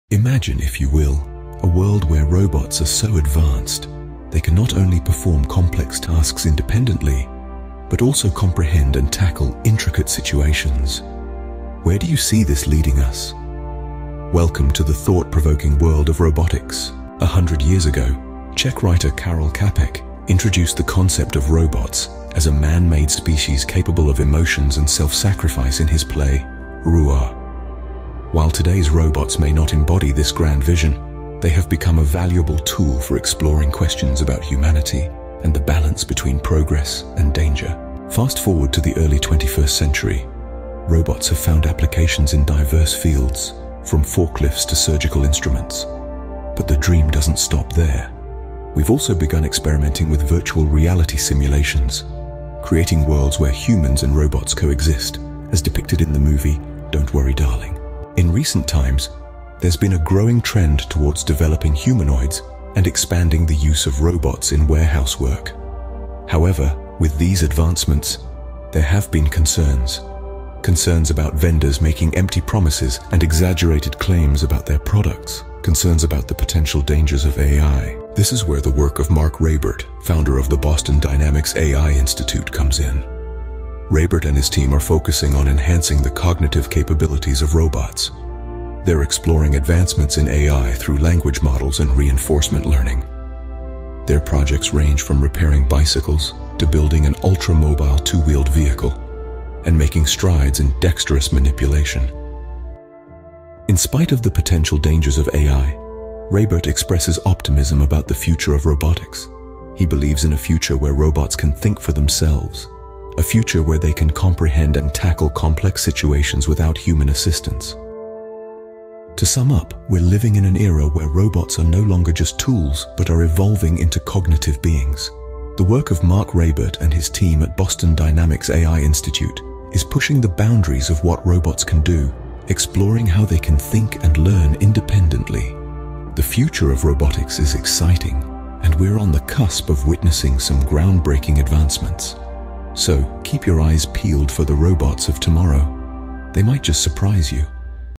AI News | Boston Dynamic's robots: The Next Cognitive Leap
Welcome to AI Explorers, your gateway to the fascinating world of artificial intelligence and robotics! In this enlightening video titled "Boston Dynamic's Robots: The Next Cognitive Leap," we embark on a journey through the evolution of robotics, exploring their profound impact on various fields, and delving into the visionary insights of Marc Raibert.

🤖 Chapters 🤖
00:00:00 - Introduction to the World of Robotics
00:00:17 - The Vision of Robots in Literature
00:00:37 - Robots in Various Fields
00:00:58 - Advancements and Concerns
00:01:18 - Marc Raibert and Cognitive Robotics
00:01:42 - Optimism for the Future
00:01:57 - The Evolving Nature of Robots

Join us on this exciting journey as we unravel the potential of Boston Dynamics' robots and the next cognitive leap in the field of robotics.